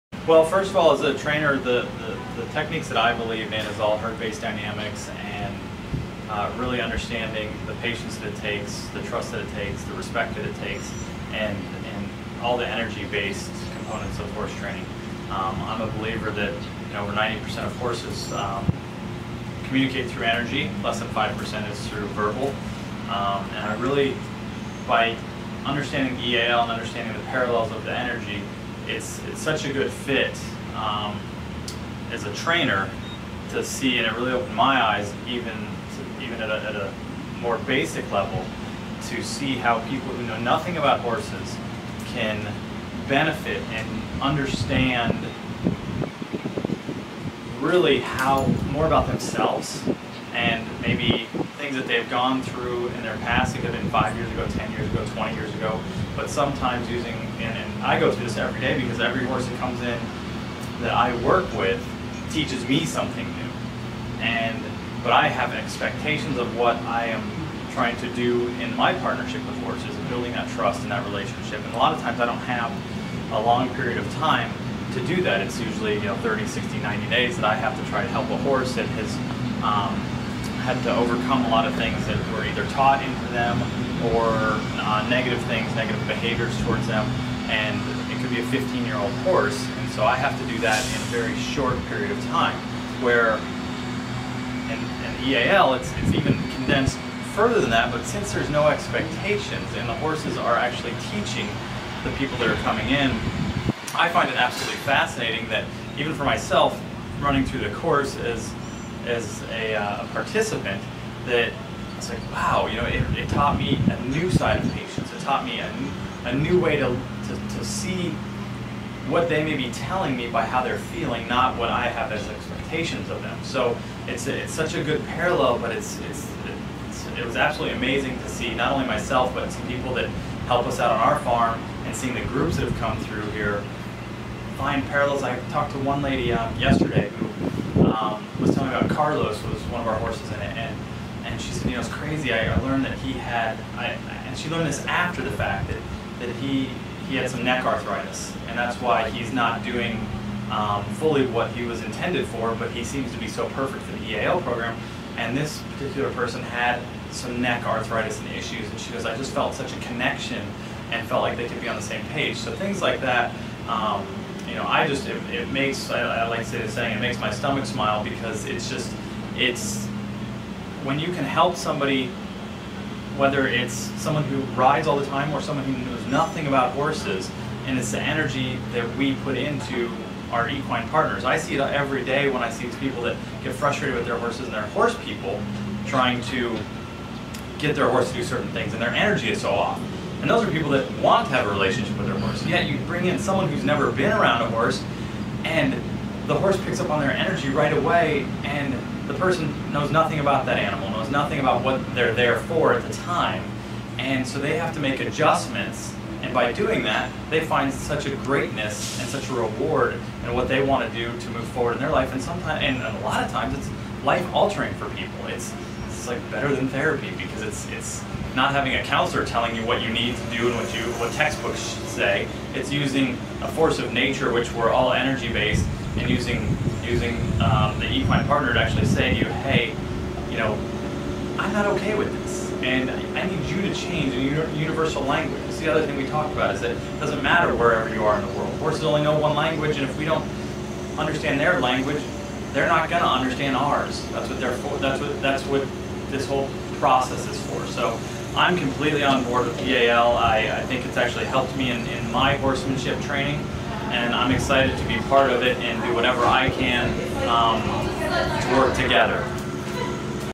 Equine Connection - The Academy of Equine Assisted Learning Inc
Horses helping people globally around the world through team building, youth experiential programs, and women’s workshops.